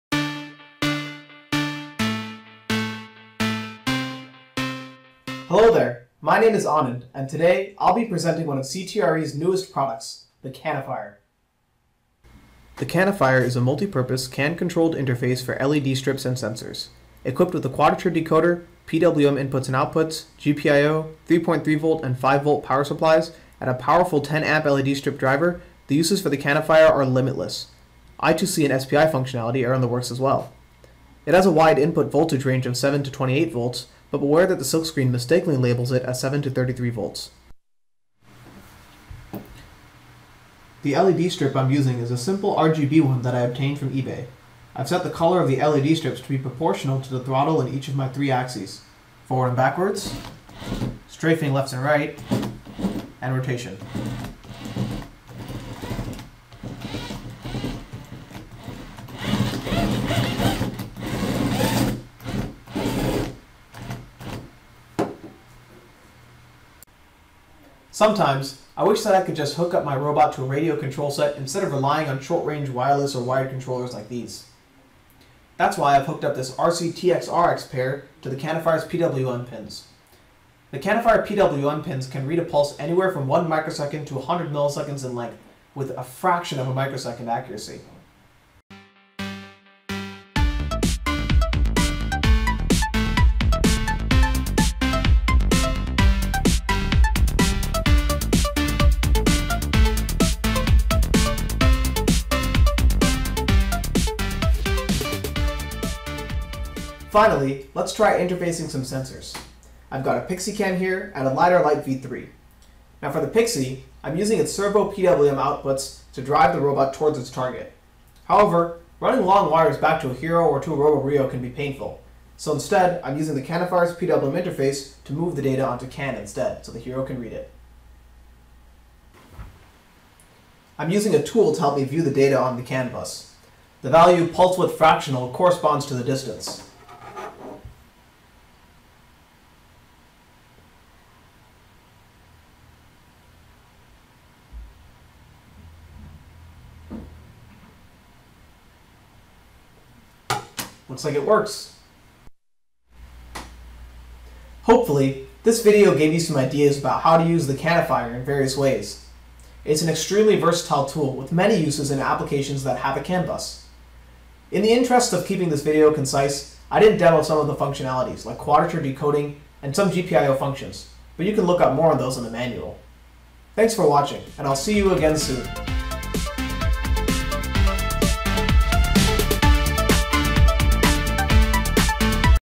💡 What Is the CANifier? CAN-to-IO Converter with RGB LED Control for FRC Robots | CTR Electronics
The CANifier from CTR Electronics is a powerful and flexible CAN-to-IO converter designed for FRC and advanced robotics applications. With support for PWM input/output, quadrature decoding, and GPIO, it bridges the gap between CAN-based systems and external devices—while also powering up to 10 amps of RGB LED strips for visual feedback and robot signaling.

🔧 Key Features:
CAN-to-IO interface for flexible signal routing
Powerful 10A RGB LED driver
Supports PWM inputs/outputs
Quadrature decoding for encoder signals
Multiple GPIO (General Purpose I/O) channels
Field-upgradeable firmware for future-ready performance
Perfect for use with the Phoenix software ecosystem and CAN-based FRC control systems.